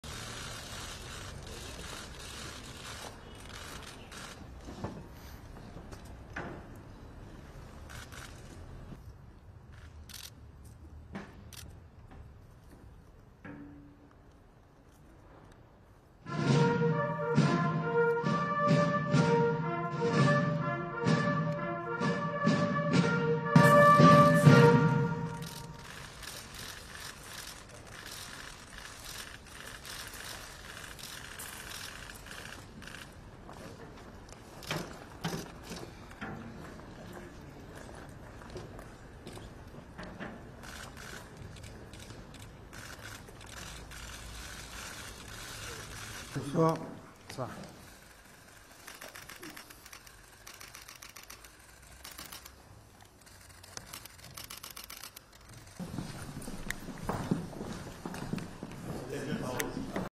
Macron welcomes Finnish counterpart Sauli Niinisto
(21 Mar 2022) French President Emmanuel Macron welcomed his Finnish counterpart Sauli Niinisto at the Elysee Palace on Monday.
According to the Finnish presidency website, the pair were to discuss Russia's invasion of Ukraine, and the security situation in Europe.